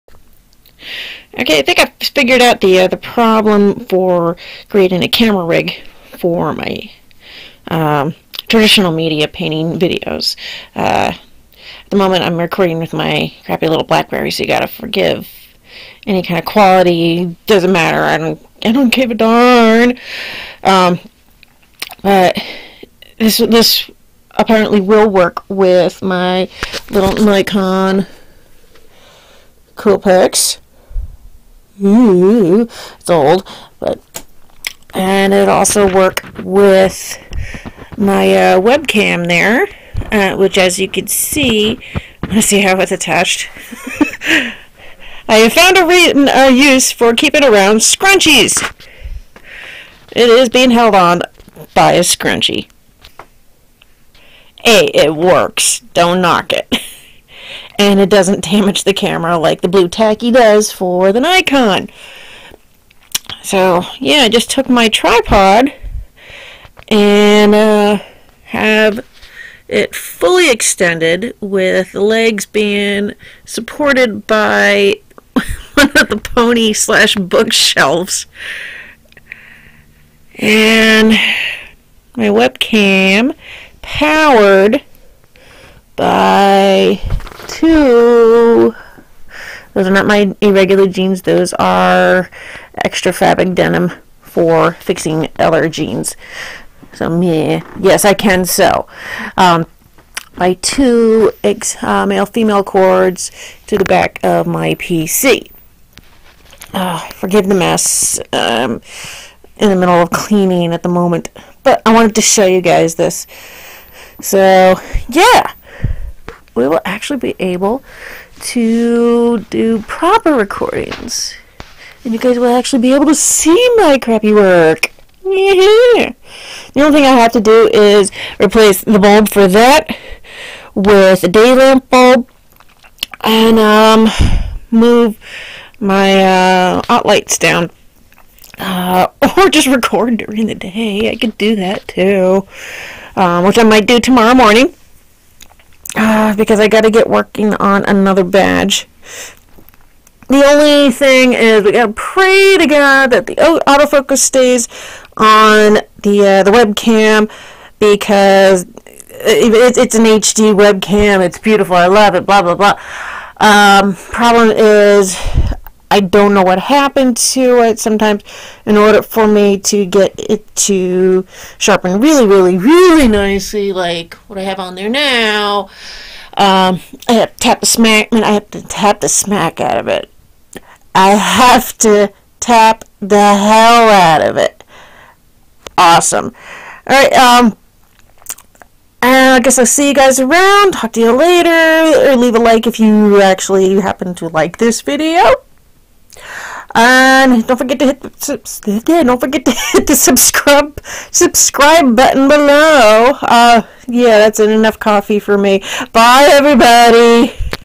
The camera rig of Bee
So I FINALLY came up with a camera rig for when I do my traditional medium art videos! Yeah, I've been looking for ways to set up SOME kind of rigging to do down shots on my angled art desk (I keep my art desk tilted high, as it is easier on my back, and I have a lesser chance of my artwork looked skewed / the perspective being off.).

But pretty much EVERY how to video I've come across here on Youtube was for flat desk set ups, or for people who can screw things into their ceilings. Which I can't.

So I finally put two and two together, pulled out my tripod, extended the legs all of the way, hung my webcamera from it, and voila! instant down shot for angled art desks camera rig! YAY!

And to celebrate this victory, I thought I would share this victory with you, AND see how well the video camera on my Blackberry Curve 9370 worked (yeah, I know, old phone. Hey, it still works, and I can't afford to get anything new and shiny right now.).

You also get to see just HOW I keep my webcam held on to the tripod every time, since it's not one with a built in clip. It's meant to hang off of the top of a flat screen computer monitor. See, scrunchies can be used for things other than hair!